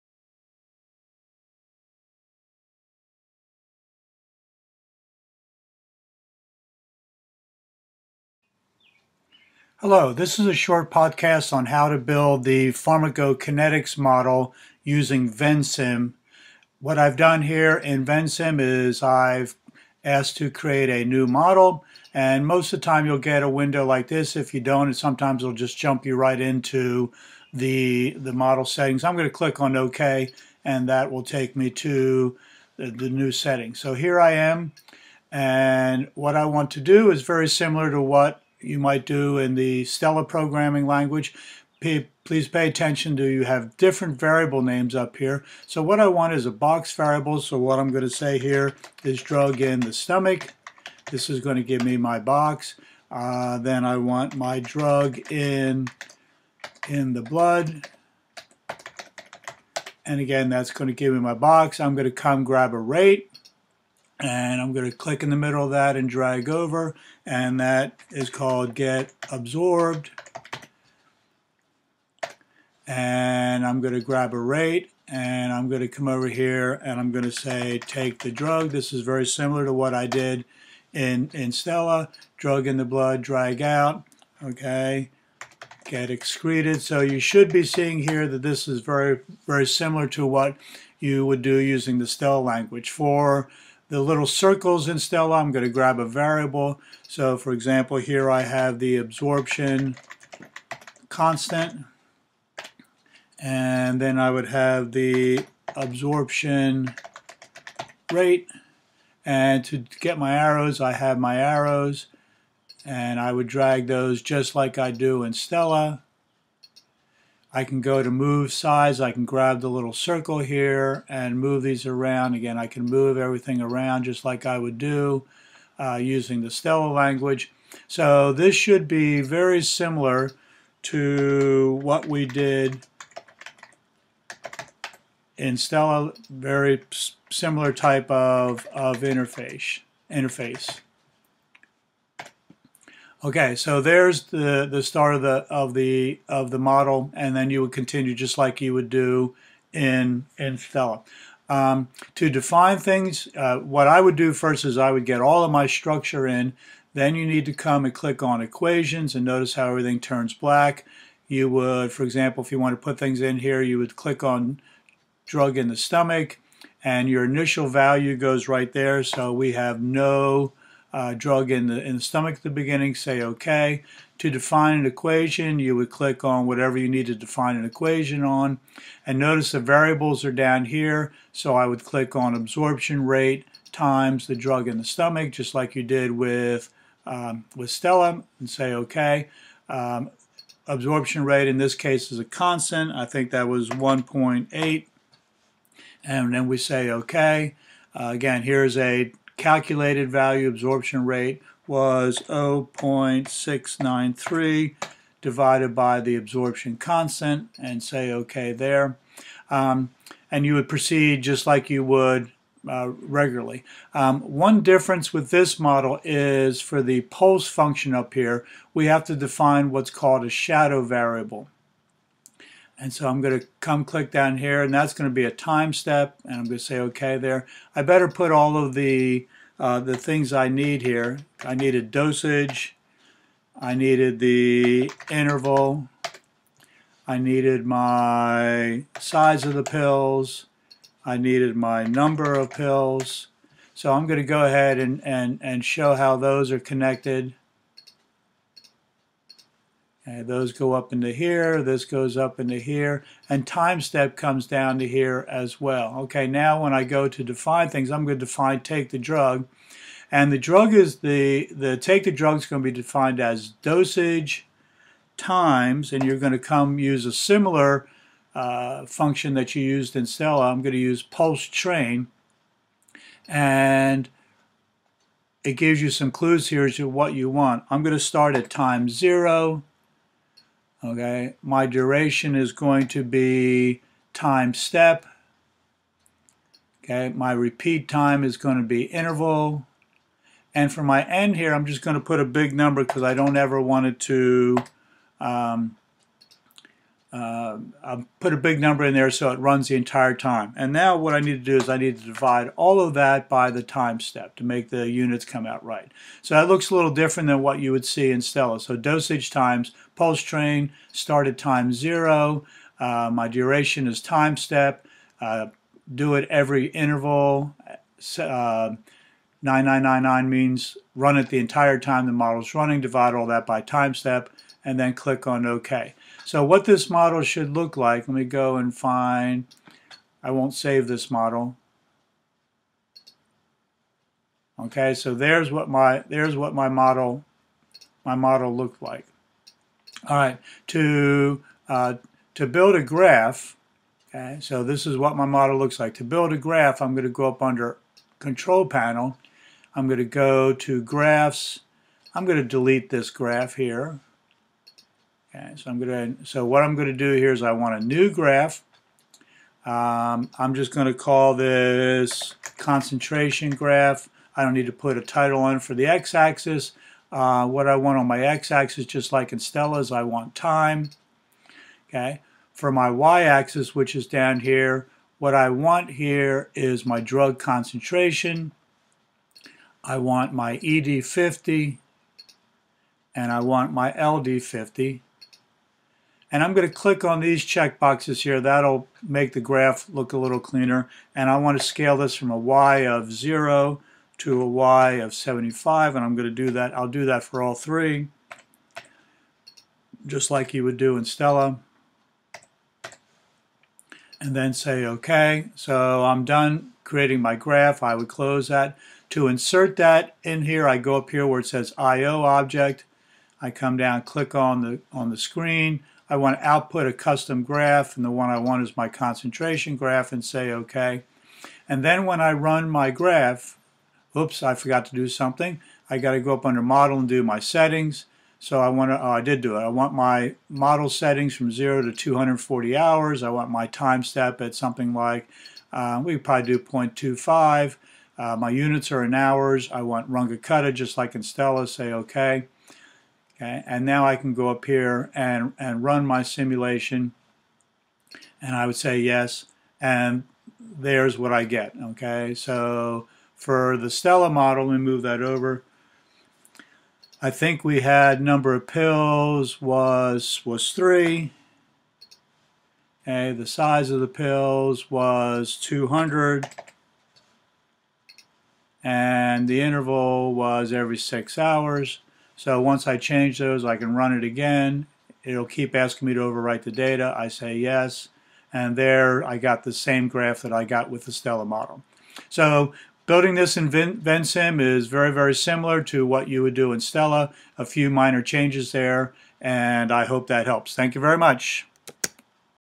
PK Model
Very short podcast to show STELLA users how to build the pharmacokinetics model using VenSim